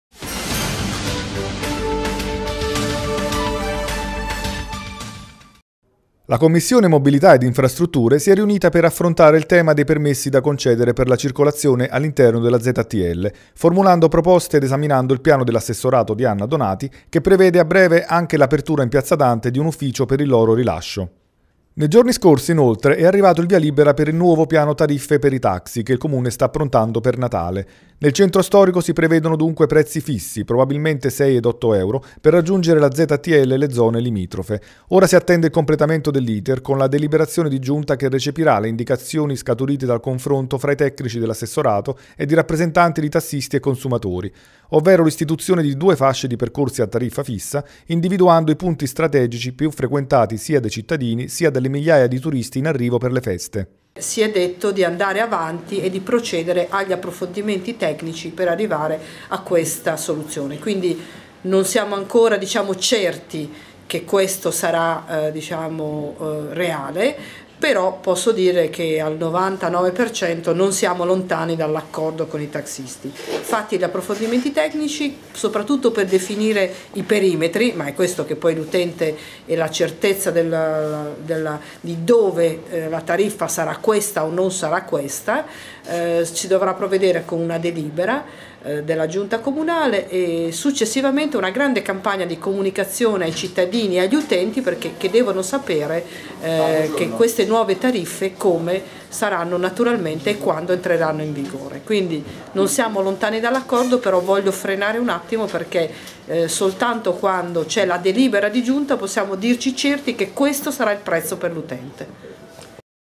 6 ed 8 €: tariffe fisse per i taxi nella ZTL
Mentre in commissione mobilità ed infrastrutture si affronta il tema dei permessi per la circolazione nella ZTL, arriva il via libera per il nuovo Piano Tariffe per i taxi: ci saranno due fasce a prezzo fisso per raggiungere la Ztl e le zone limitrofe nei punti strategici più frequentati, sia dai cittadini che dai turisti in arrivo per le festività.